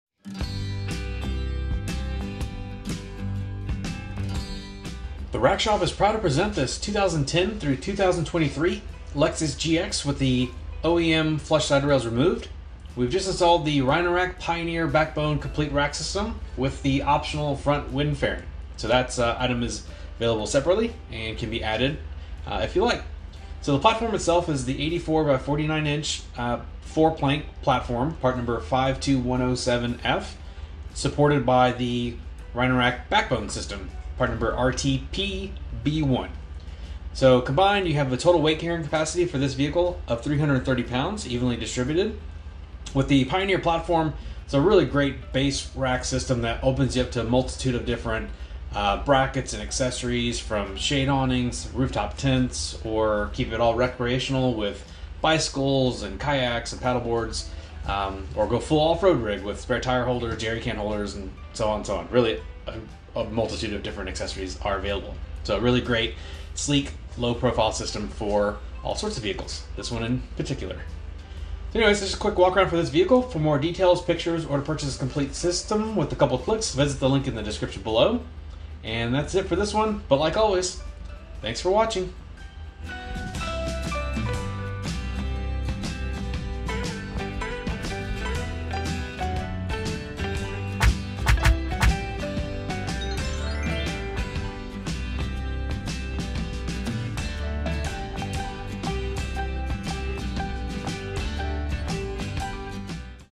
2010-2023 Lexus GX 460 with Rhino-Rack Backbone & Pioneer Platform Roof Rack | The Rack Shop
A quick video of the Rhino-Rack Pioneer Platform and Backbone roof rack system on a Lexus GX 460 with the OEM Flush Rails Removed.

for more details, pricing, and easy ordering.

Have more questions? No problem! Email, call, or swing on by the shop!